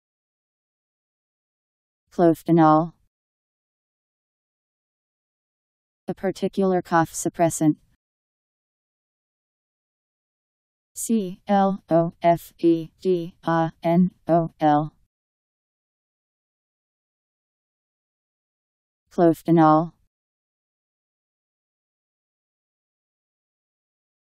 Clofedanol Meaning
Video shows what clofedanol means. A particular cough suppressant..  Clofedanol Meaning. How to pronounce, definition audio dictionary. How to say clofedanol. Powered by MaryTTS, Wiktionary